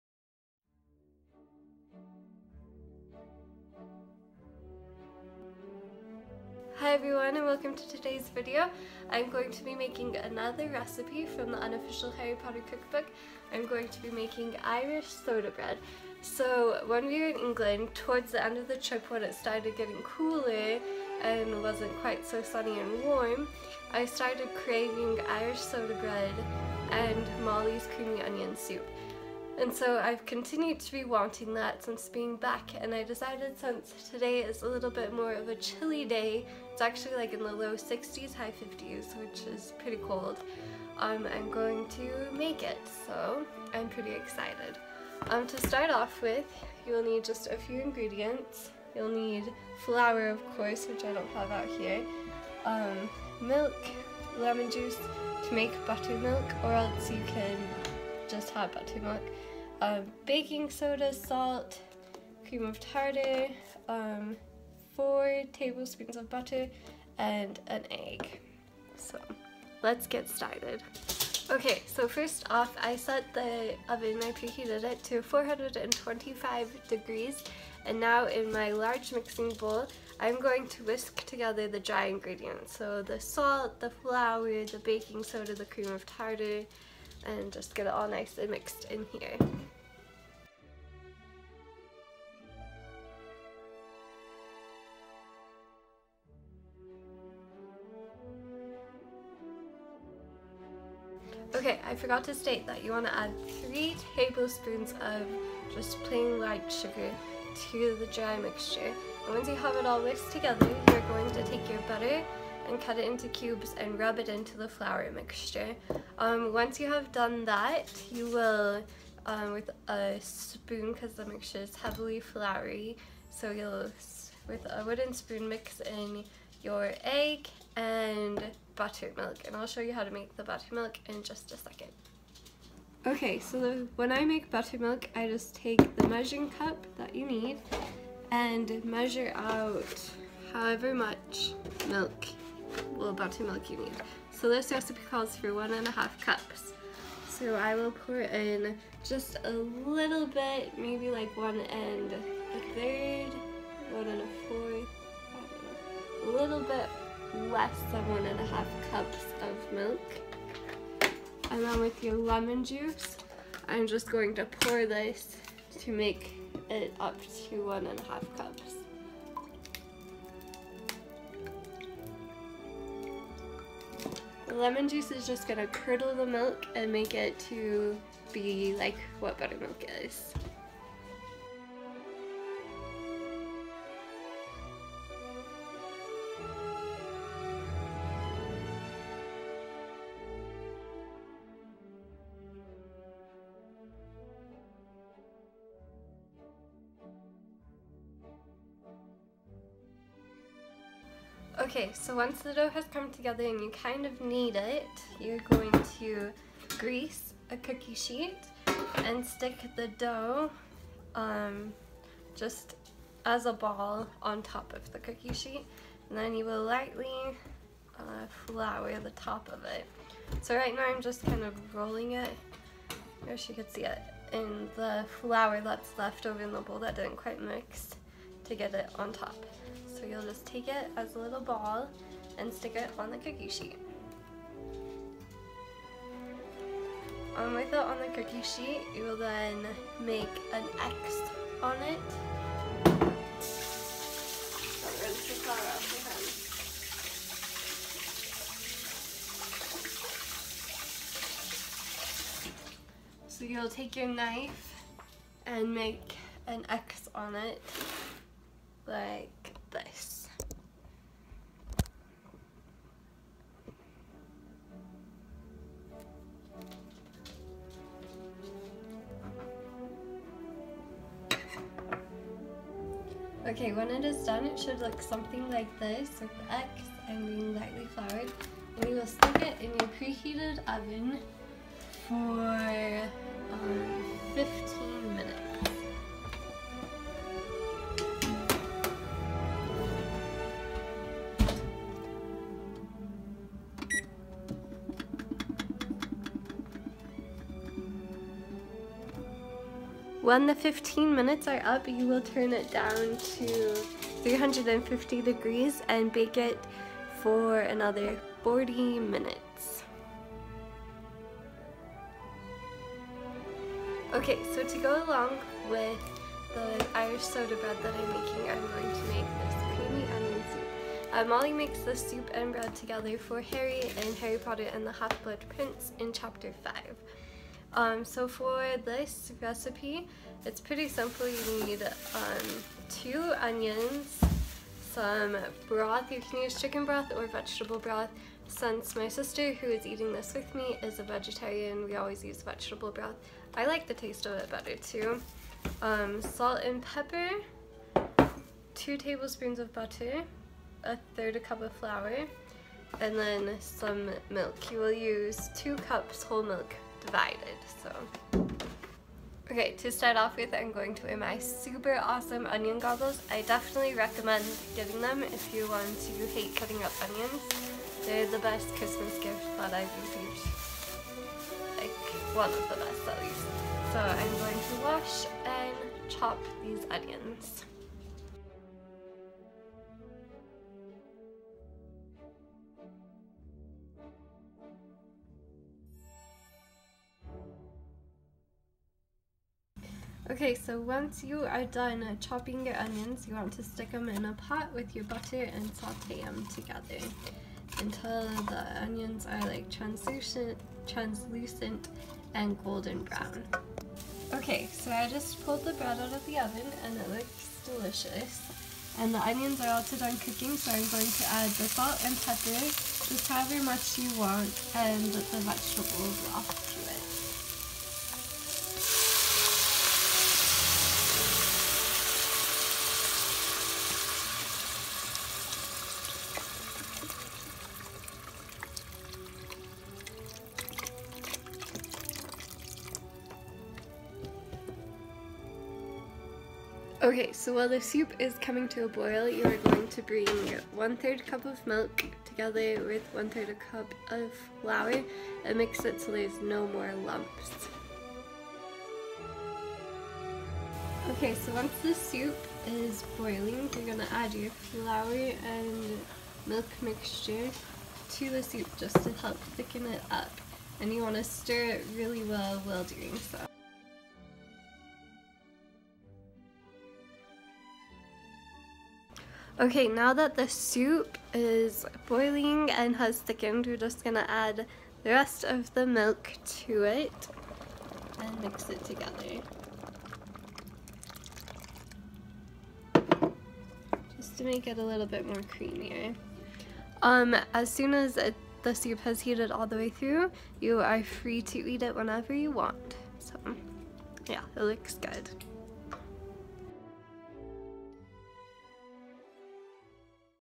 Harry Potter Cooking Irish Soda Bread And Creamy Onion Soup
Olivia makes another recipe from the Unofficial Harry Potter Cookbook. I hope you enjoy!